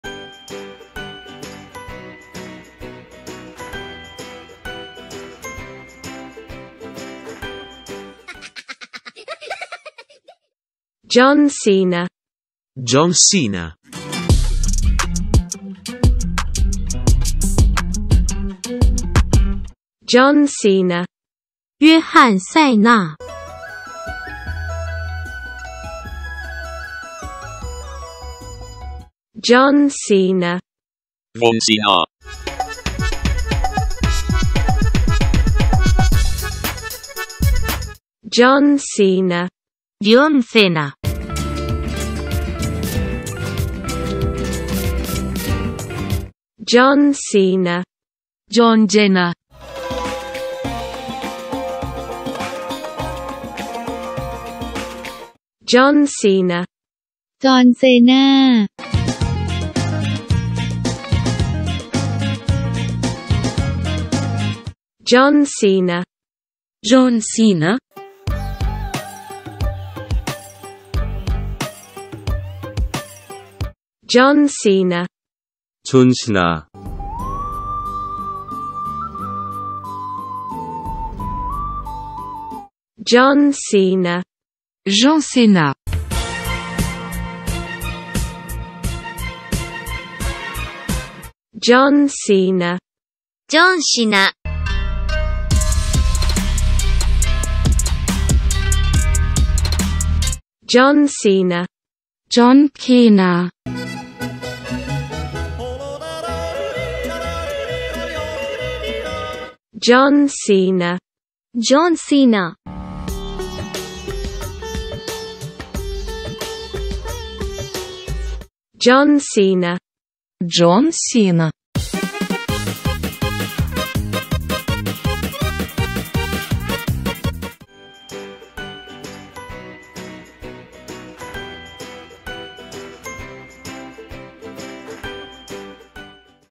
John Cena in different languages meme
ITALIAN 0:10
CHINESE (TRADITIONAL) 0:19
ARMENIAN 0:29
SPANISH 0:38
TURKISH 0:46
THAI 0:55
ARABIC 1:04
KOREAN 1:13
FRENCH 1:23
JAPANESE 1:31
GERMAN 1:40
HINDI 1:49
UKRAINIAN 1:58